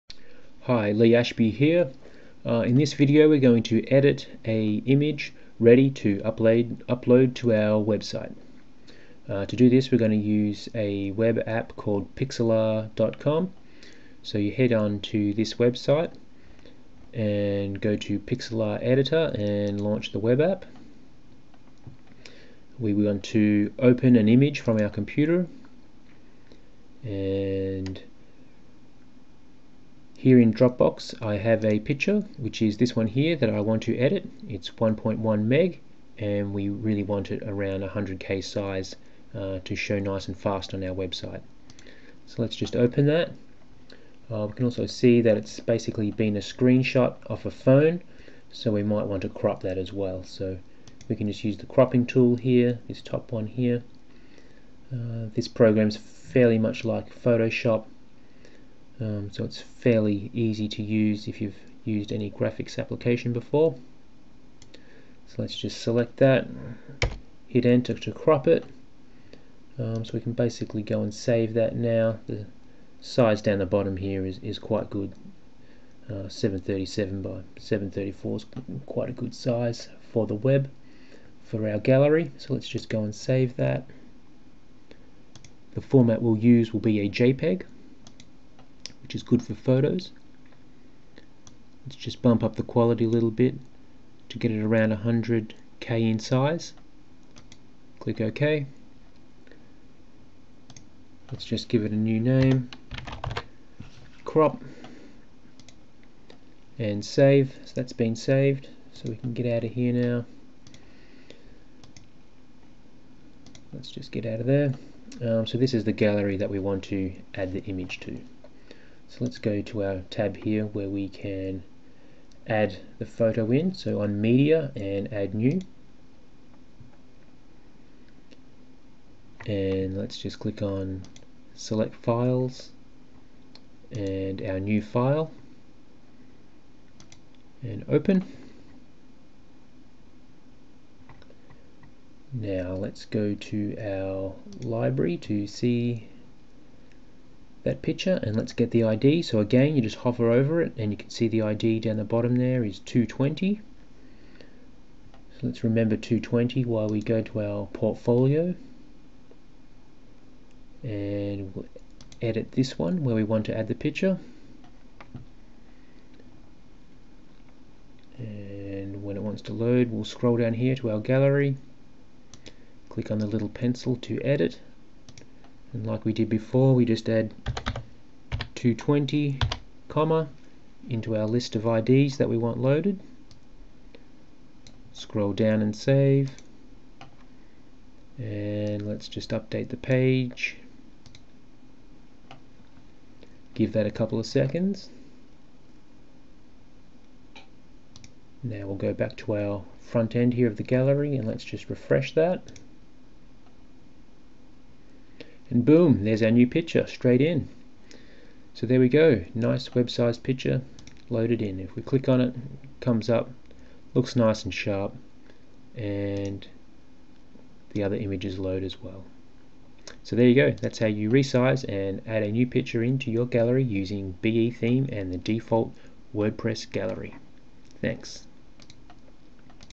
Crop and Reduce Picture Size For Display in BeTheme Wordpress Gallery
Use Pixlr.com to crop and reduce the file size of a picture ready for uploading to your website.